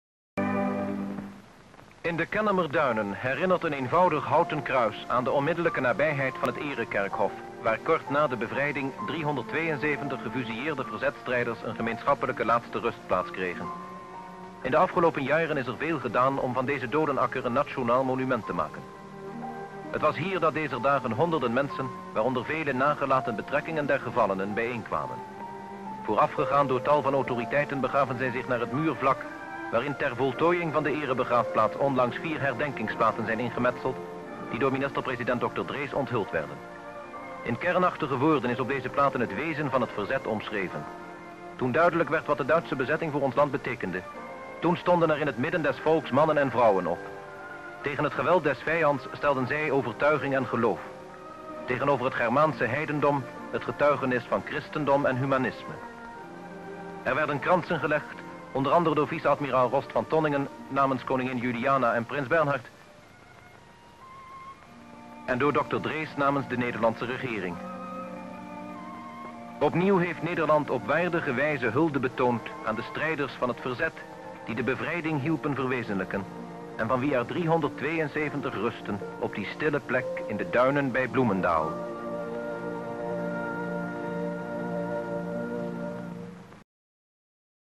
Unveiling of commemorative plaques at the honorary cemetery (Week number 53-45) 🔴
Description (more): At the honorary cemetery for executed resistance fighters from World War II in the Kennemer dunes commemorative plaques are unveiled. Wreath layings by Prime Minister dr. Willem Drees sr. on behalf of the governement and vice-admiral Rost van Tonningen on behalf of Queen Juliana and Prince Bernhard.